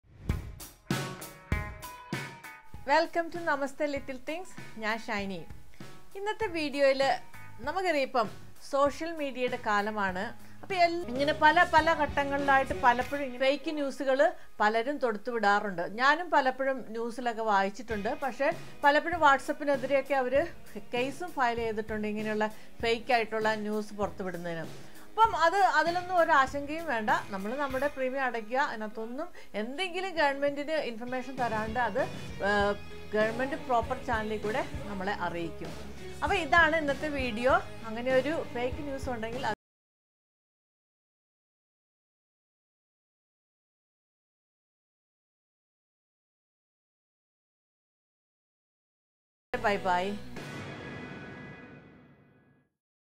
Fake news about LIC policy /നിങ്ങൾ LIC പോളിസി holder ആണോ വീഡിയോ കണ്ടു നോക്കു LIC India#
Fake news regarding LIC policy spreading in what’s App. Check the reality in the video